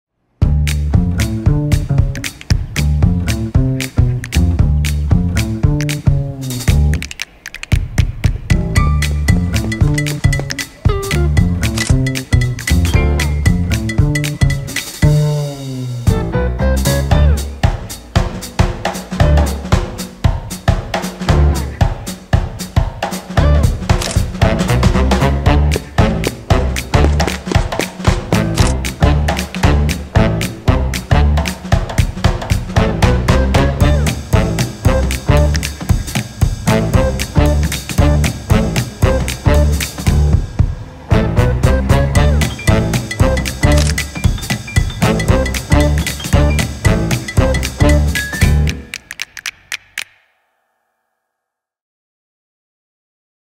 SAMSUNG I Ustvarite popolno fotografijo z Galaxy Watch4
Shranite vse popolne posnetke in izkoristite zaslon vaše nove ure Samsung Galaxy Watch4, da vidite enako kot kamera vašega telefona, z odlično možnostjo Camera Controller! Izvedite več